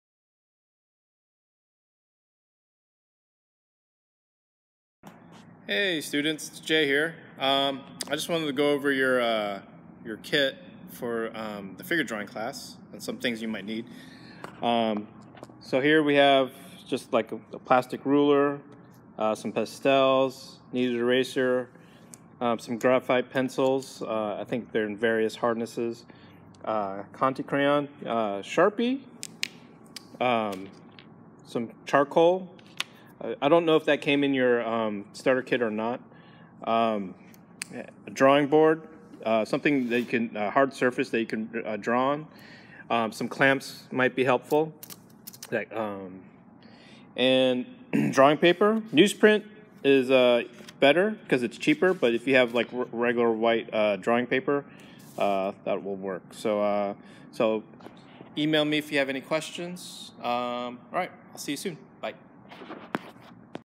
Material List
These are the materials you will need for the figure drawing class